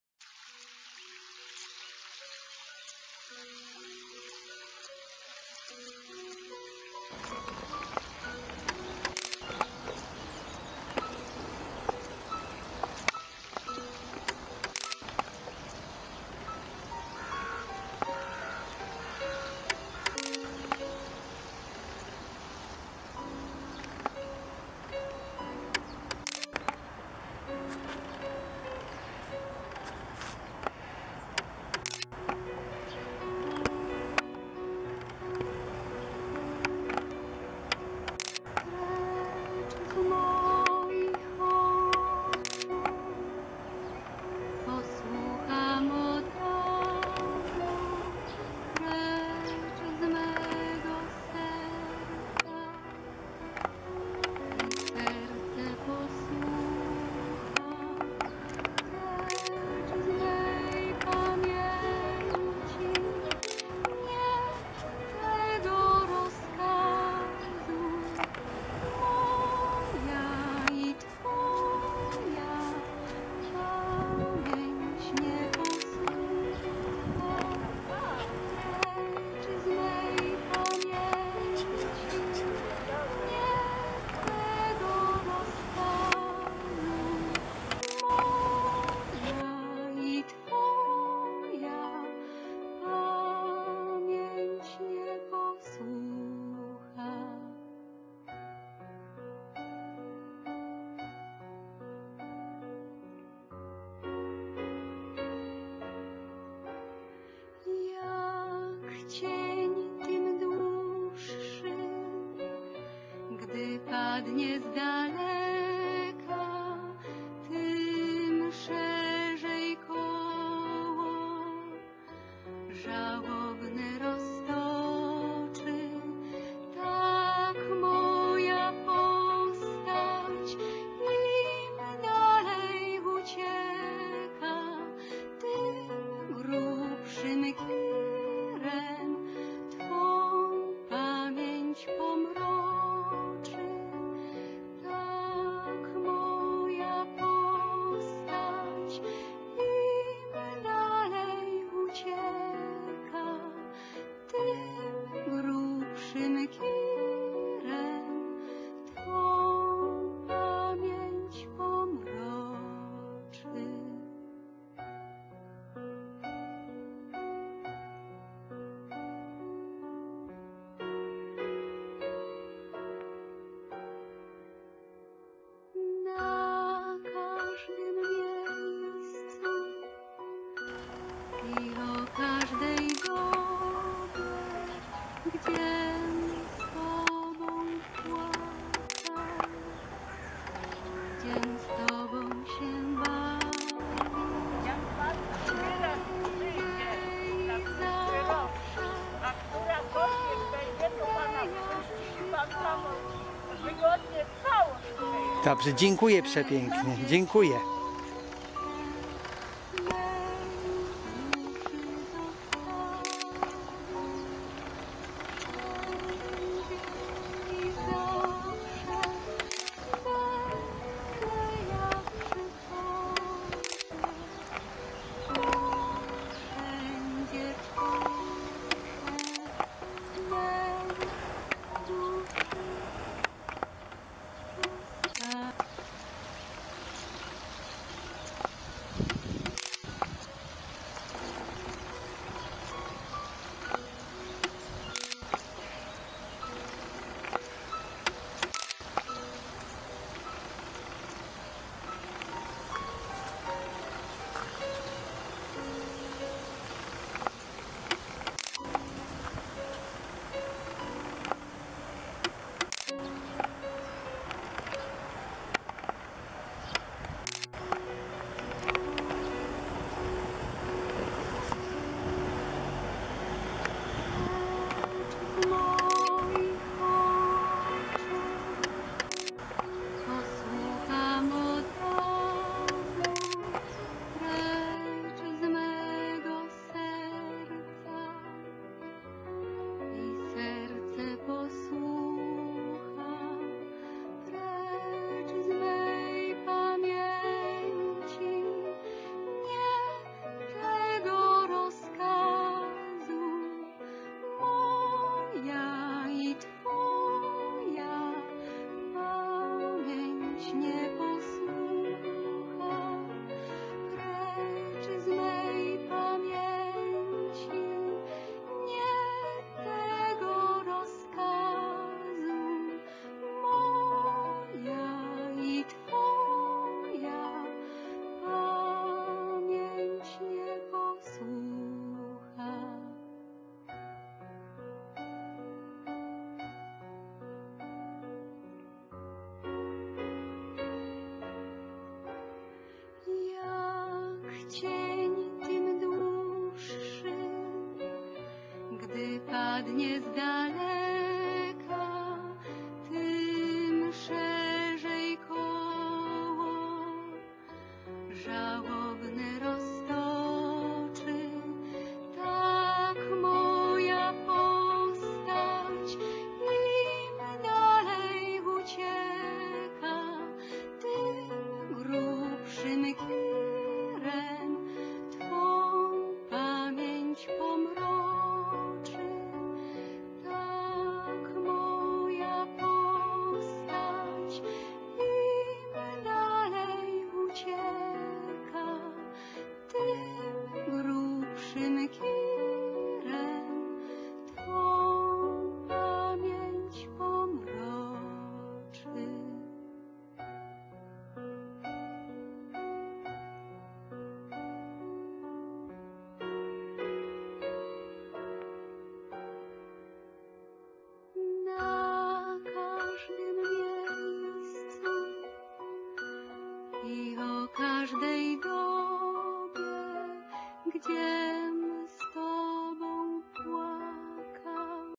2021 10 20 Warszawa Spacerując po ulicy Astronautów Groszek ogrodowy zakwitł po raz trzeci w tym rok
Spacerując po ulicy Astronautów / Strolling down Astronautów Street / En flânant dans la rue d'Astronautów
Groszek ogrodowy zakwitł po raz trzeci w tym roku. Pogodowe anomalie. / Garden peas have bloomed for the third time this year. / Les pois de jardin ont fleuri pour la troisième fois cette année.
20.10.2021 po / after / après 09.52